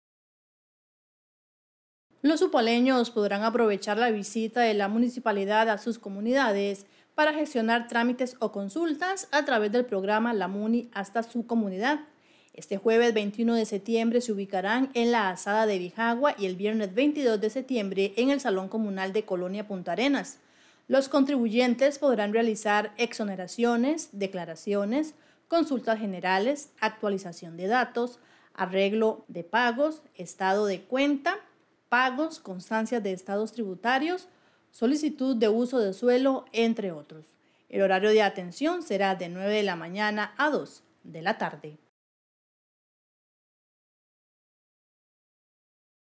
MUNI HASTA SU COMUNIDAD LLEGARÁ A BIJAGUA Y COLONIA PUNTARENAS
Los upaleños podrán aprovechar la visita de la municipalidad a sus comunidades para gestionar trámites o consultas, a través del programa la Muni hasta su comunidad